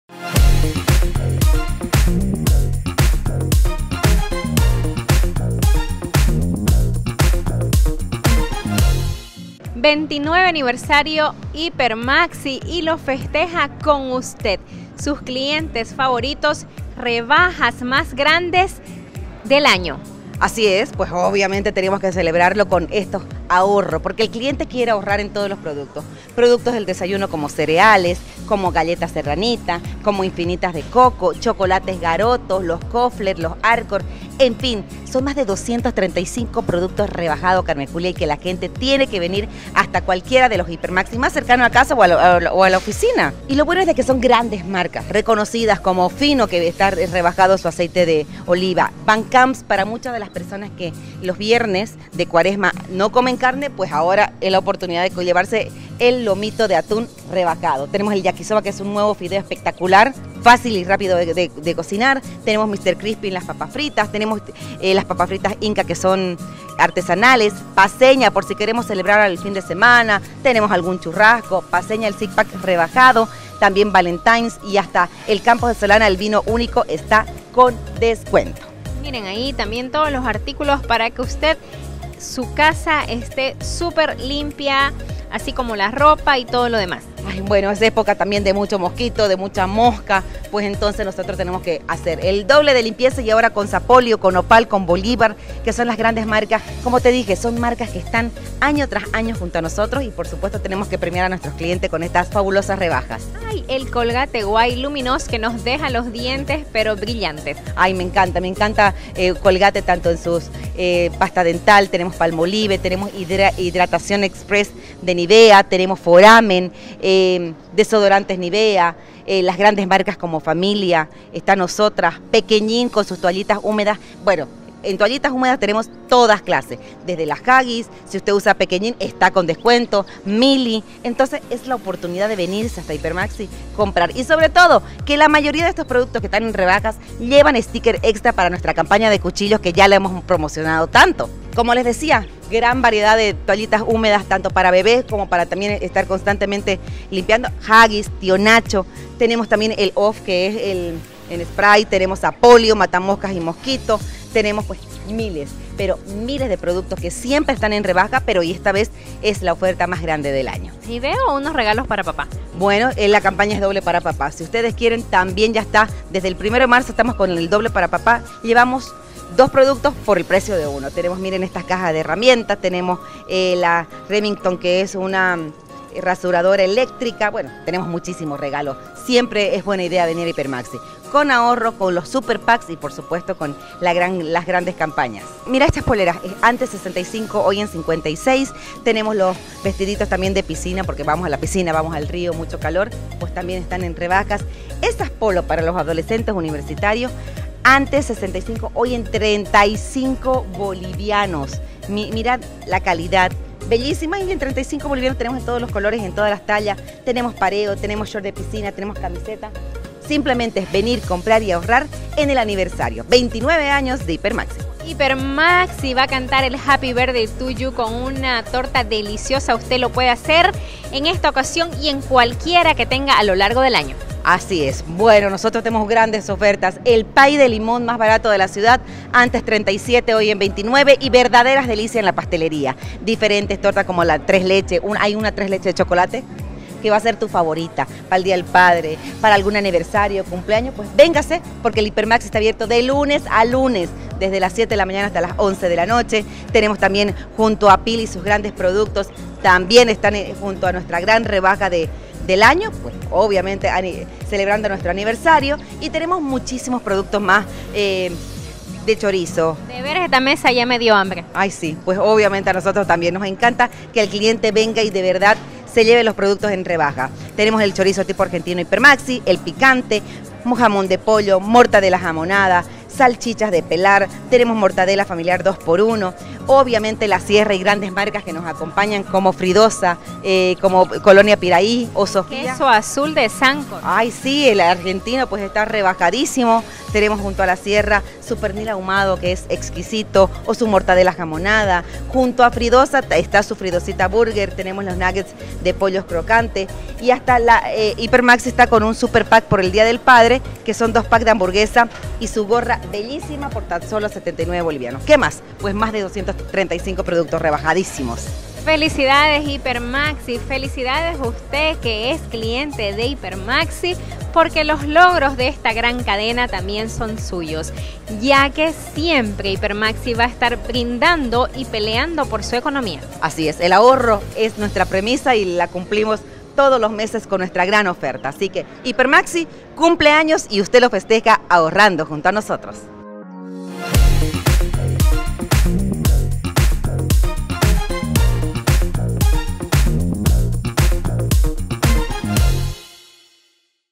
“HIPERMAXI CUMPLE 29 AÑOS” LA MEJOR OFERTA DE AHORRO DEL AÑO
En esta oportunidad HIPERMAXI por sus 29 años de aniversario cuenta con más de 235 productos con descuentos increíbles como regalo a la fidelidad de sus clientes.